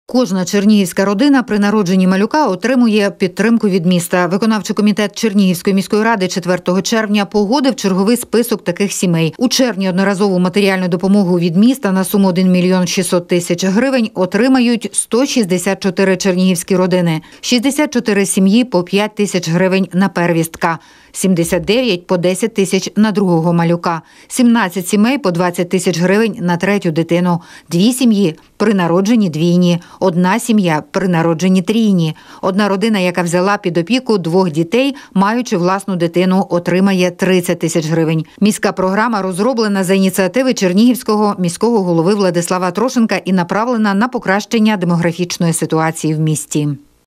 Допомога при народжені| Телеканал Новий Чернігів
Кожна чернігівська родина при народженні малюка отримує підтримку від міста. Виконавчий комітет Чернігівської міської ради 4 червня погодив черговий список таких сімей. У червні одноразову матеріальну допомогу від міста на  суму 1,6 млн. грн. отримають 164 чернігівські родини:
64 сім’ї - по 5 тис.грн на первістка;
79  - по 10 тис.грн на другого малюка;
17 сімей - по 20 тис.грн на третю дитину;
2 сім’ї при народженні двійні;
1 сім’я при народженні трійні;
1 родина, яка взяла під опіку двох дітей, маючи власну дитину, отримає 30 тис.грн.

Нагадаємо, міська Програма розроблена за ініціативи чернігівського міського голови Владислава Атрошенка, і направлена на покращення демографічної ситуації в місті.